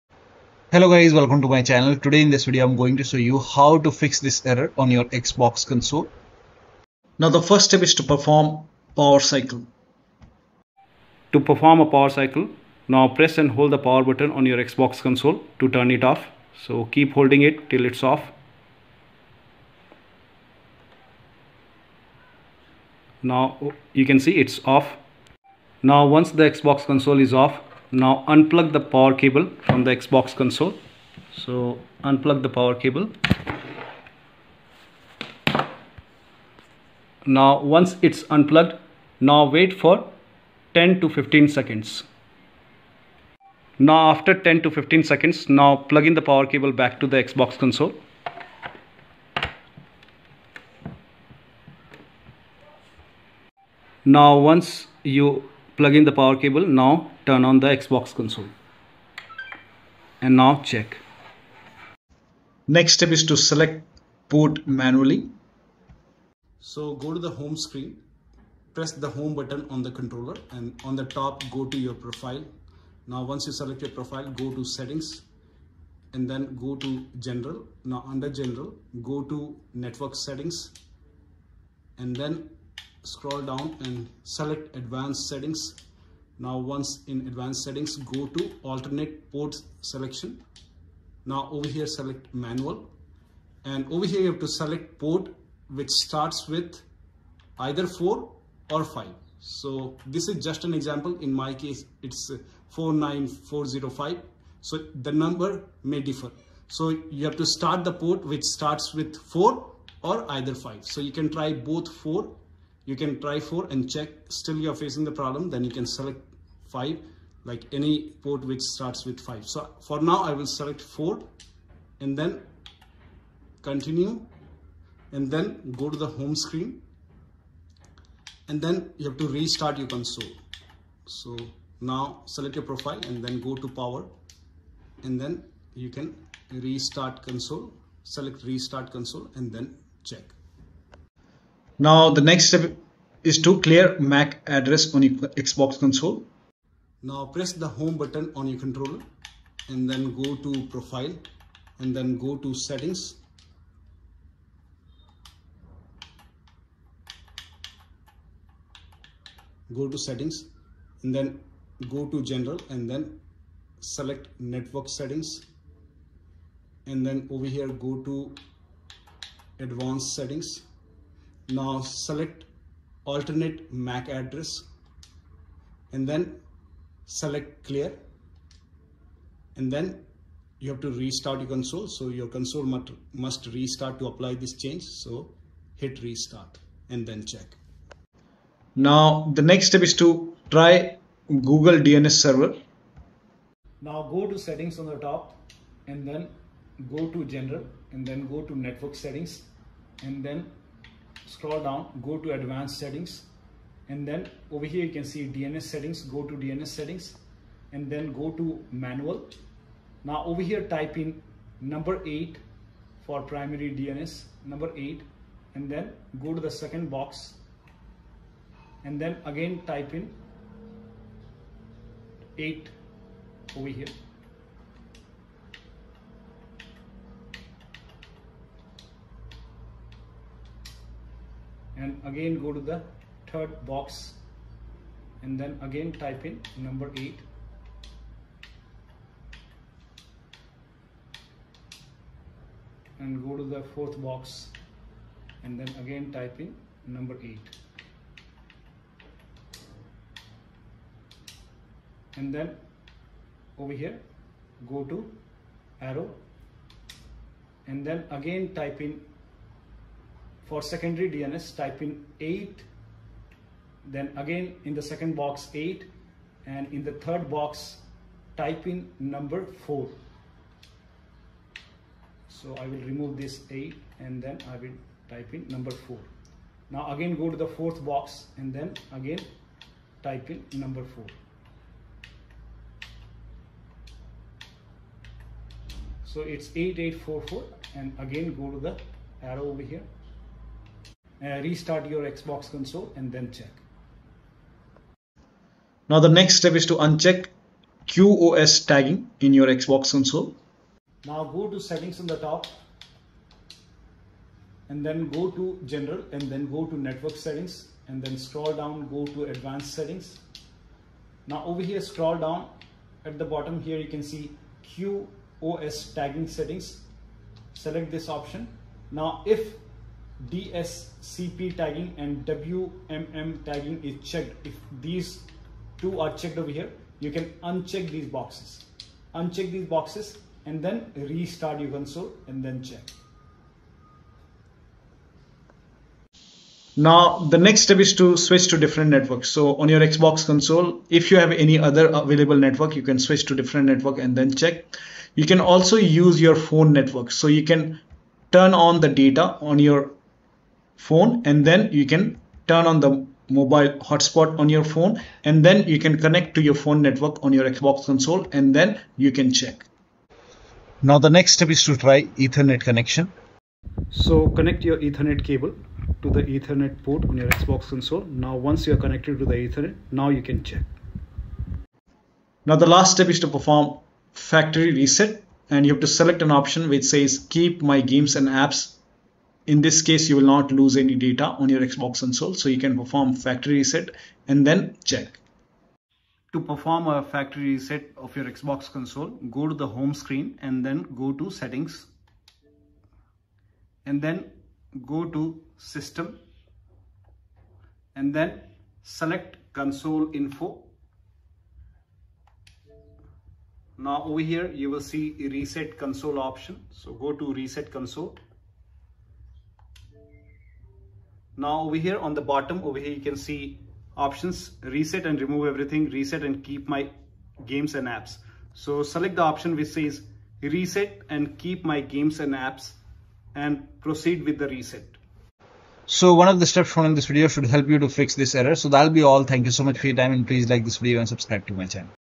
Fix Black Ops 6 HUENEME NEGEV Error On Xbox Series X/S, Fix Black Ops 6 CONNECTION FAILED ERROR XBOX
How to Fix Call of Duty Black Ops 6 HUENEME NEGEV Error On Xbox Series X/S,Fix Black Ops 6 CONNECTION FAILED ERROR XBOX